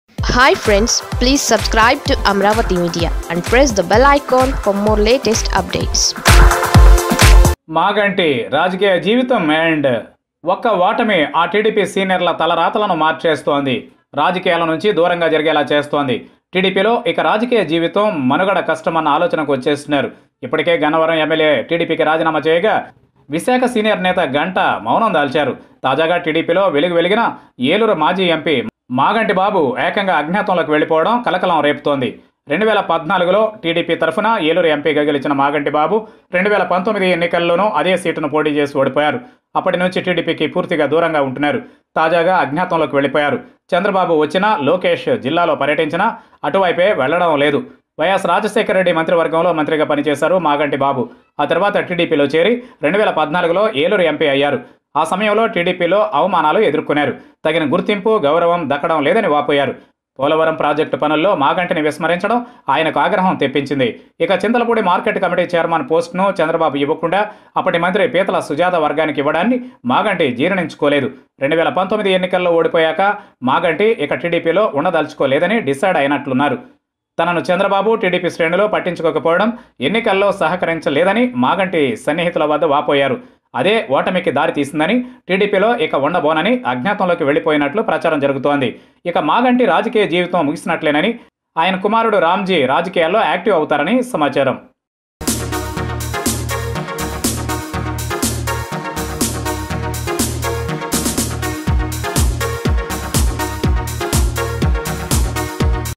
ఒకే ఒక్క ఓటమితో మాగంటి రాజకీయం ఖేల్ ఖతం..!| Ex MP Maganti Political Career End with Defeat..!
The Recent General Election has changed the fate of many TDP leaders. Many senior leaders have disappeared after losing the election. Eluru Former MP, West Godavari District Senior Politician Maganti Venkateswara Rao(Babu) one of them.